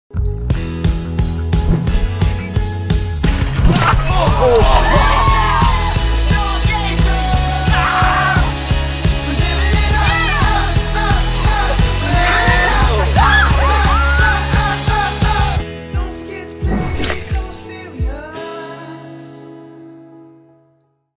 The Sweeper
You have one shot to make it across without being swept away. The Sweeper arms show no mercy and are out for vengeance, so stay alert!